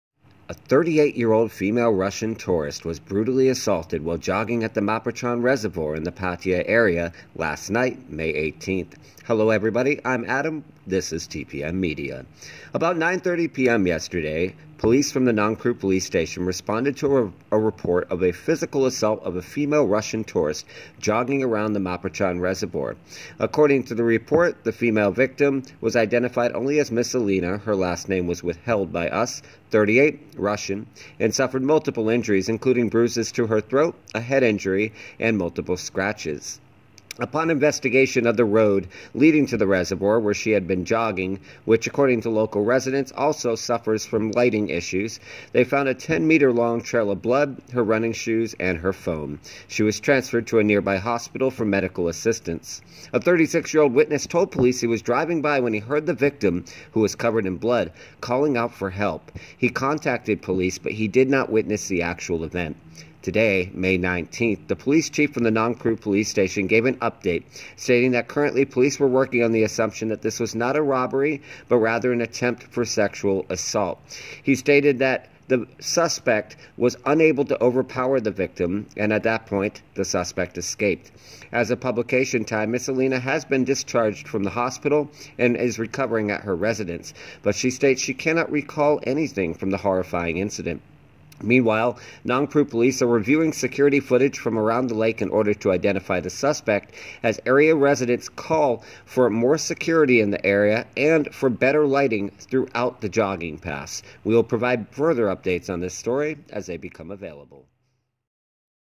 Russian tourist assaulted while jogging around reservoir in Pattaya.
OR:
Become a supporter on Facebook for plenty of extras and perks!